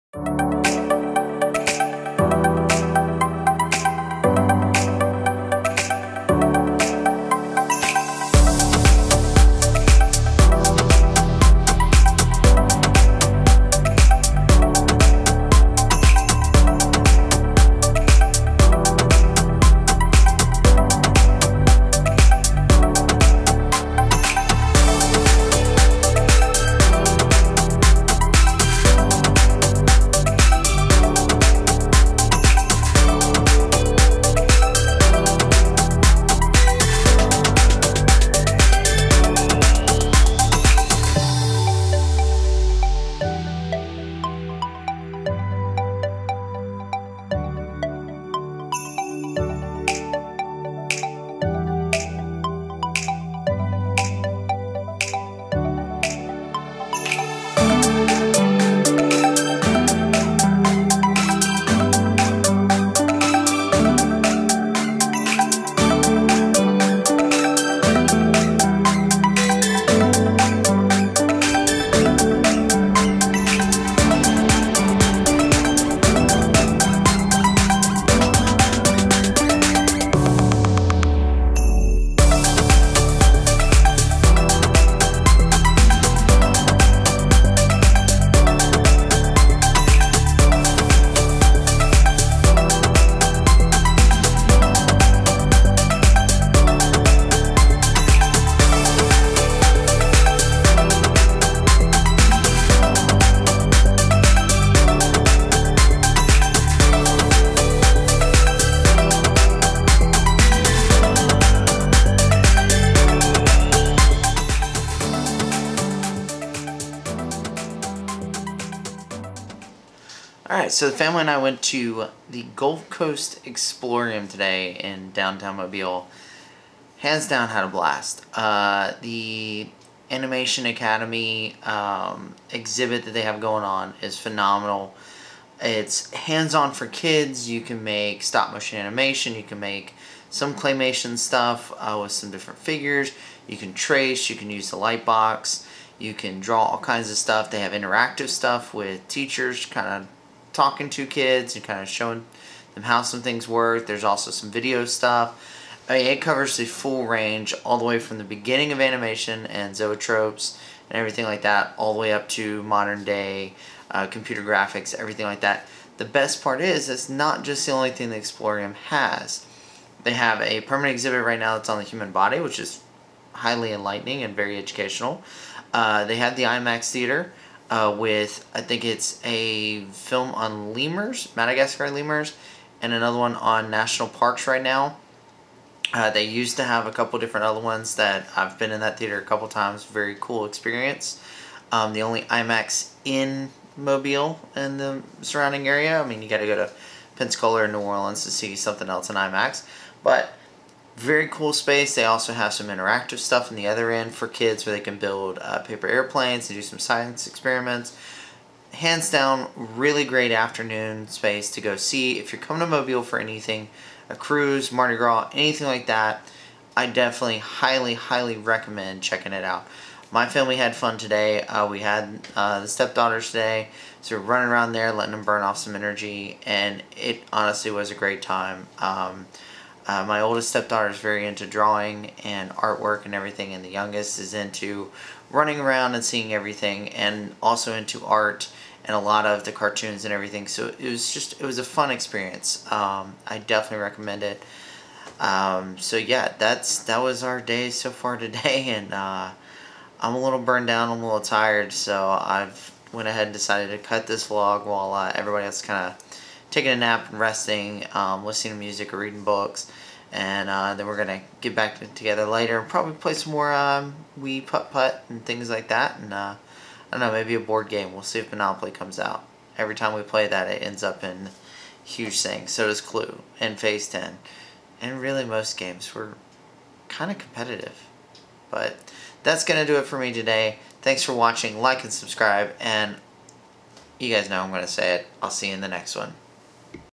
vlog s2e59 animation history overload
Toured the new exhibit at the Gulf Coast Exploreum today with the family and had a great time. The history covered in the animation academy exhibit is impressive. Definitely should go check it out!

All filmed on Galaxy S7